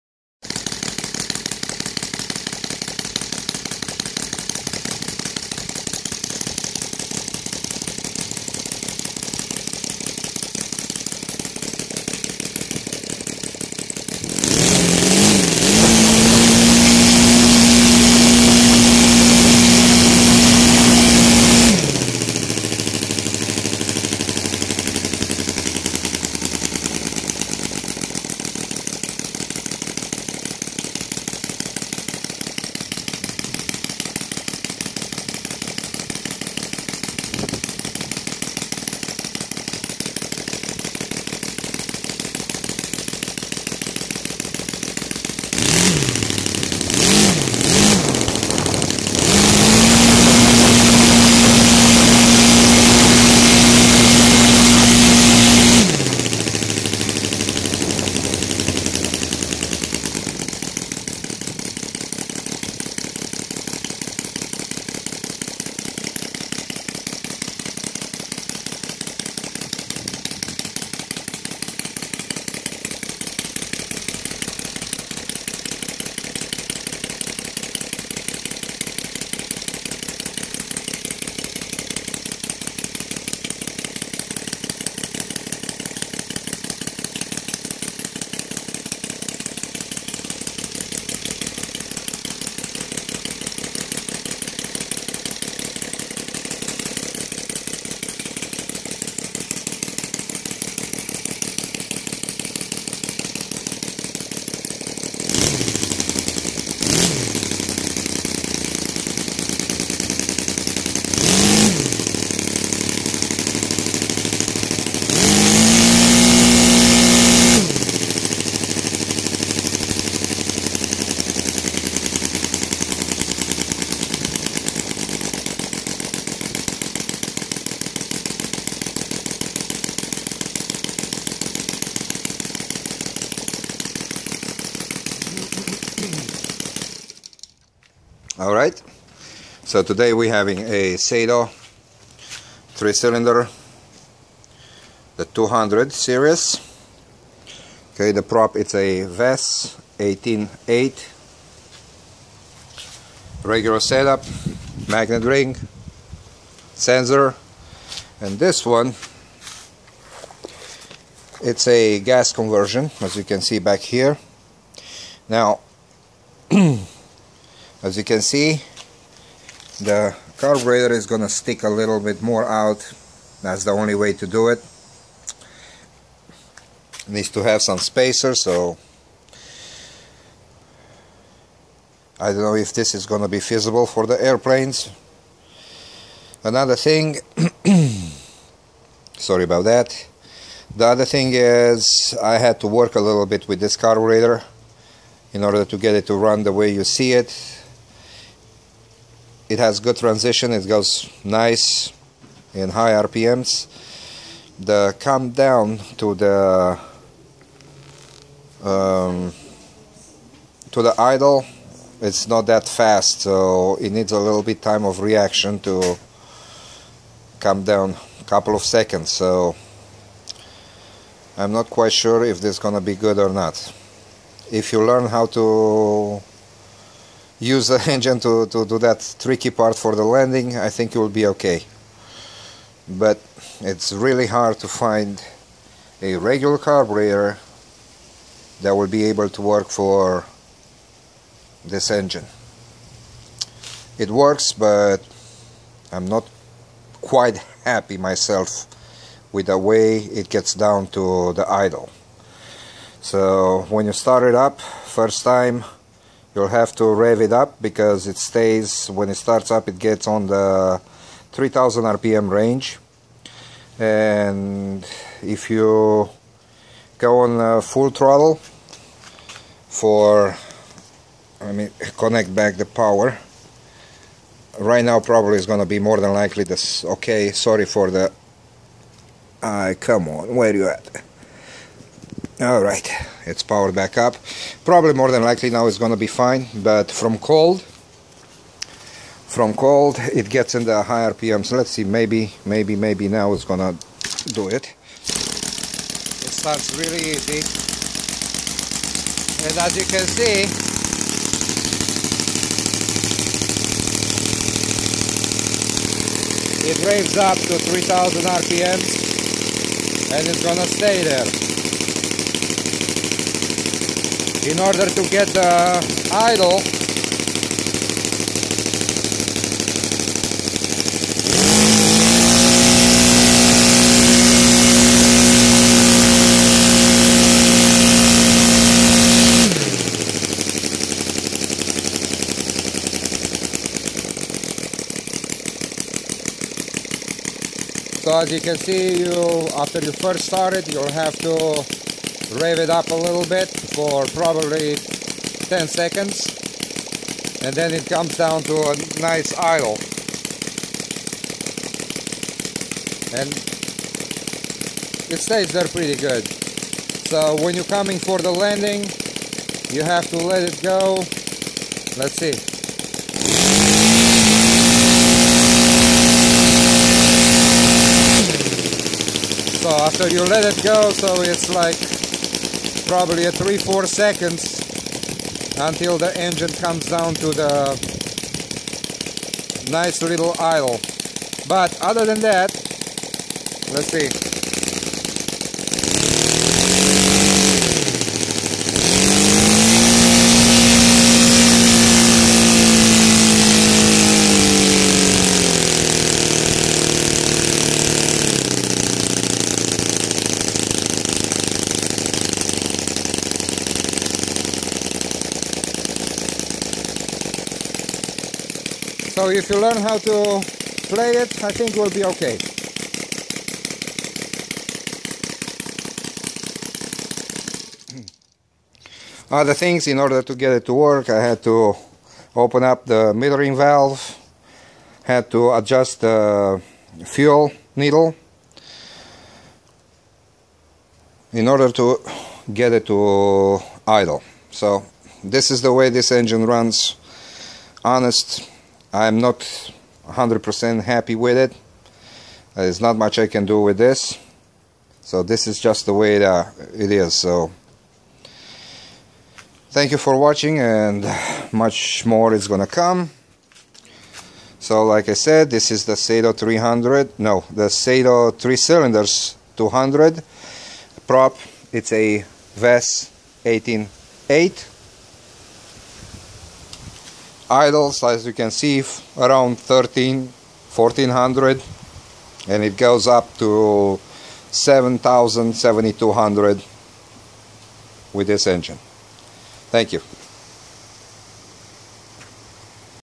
Saito Radial R200 With CH Ignition & Gasoline .
Engine converted  to gasoline.
Prop: Vess 18 x 8
Idle 1300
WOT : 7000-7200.